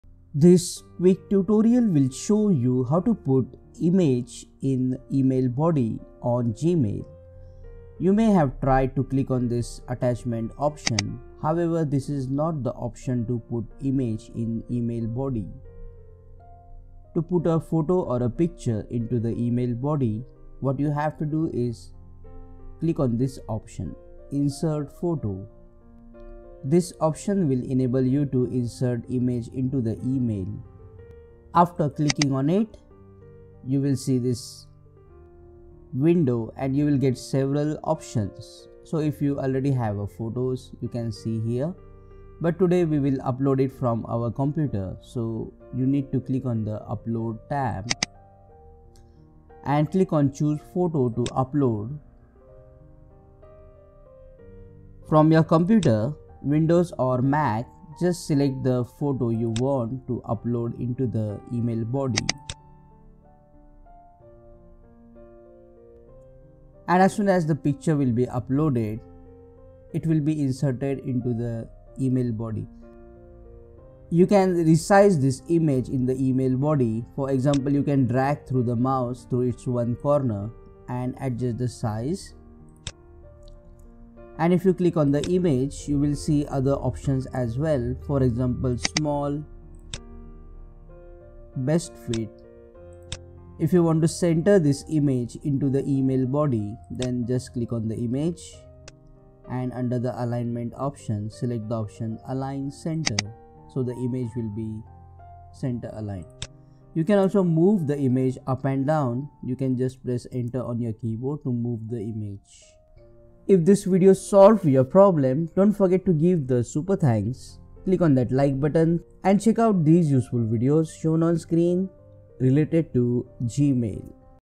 How to Put Image In Email Body - Gmail
This video will show you How to Put Image In Email Body Gmail. To add photos (Images/ Pictures) and embed them into the email using the "Insert Photo" option. This option will allow you to upload the image or multiple images from your desktop computer or Laptop (Windows or MAC). Further, you can resize the image and center align them in the email body.